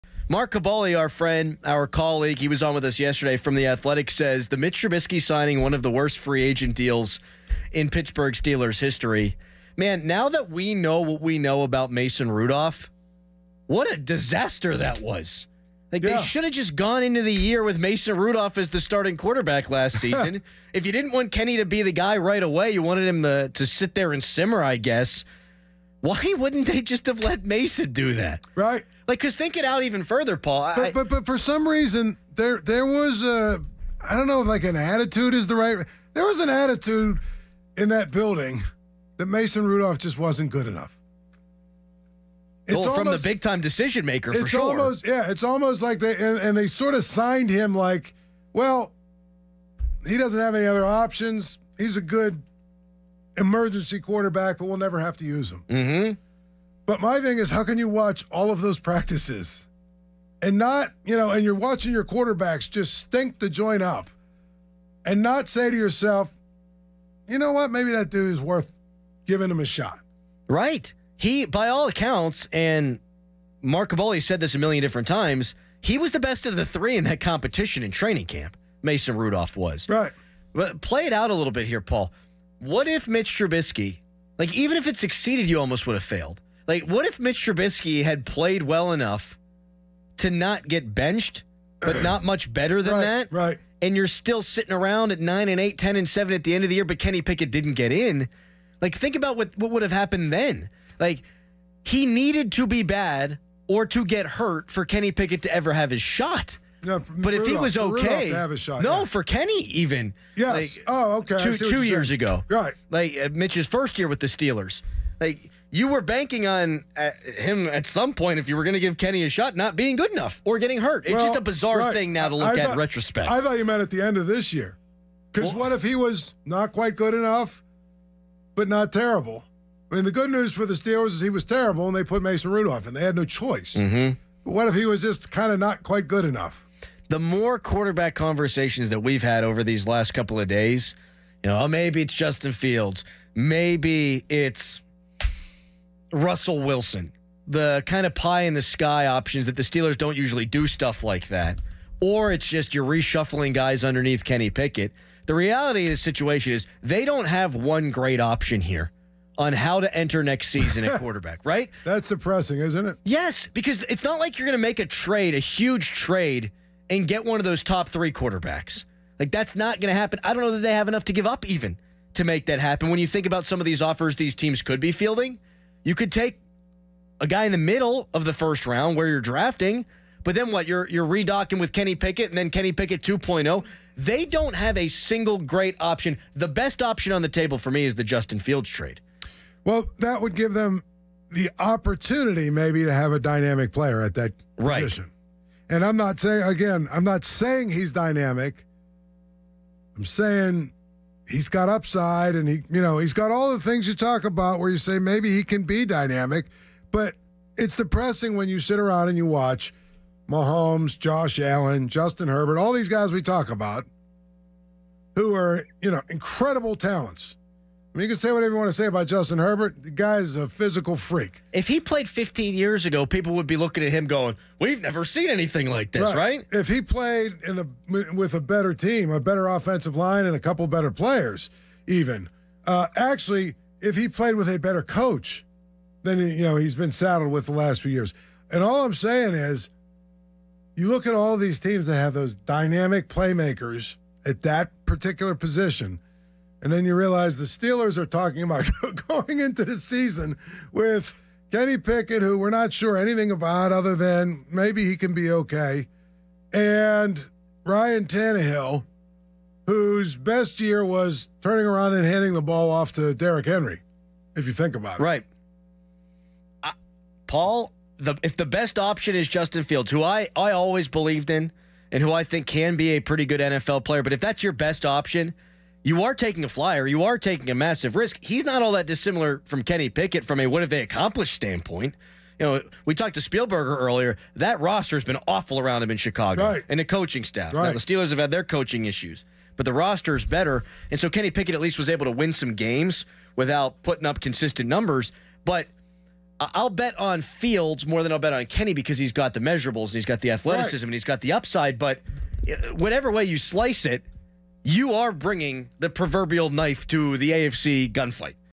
Steelers quarterback options entering the offseason
The Steelers cut Mitch Trubisky. Mason Rudolph may not return. Who's the best option at quarterback for the Steelers moving forward?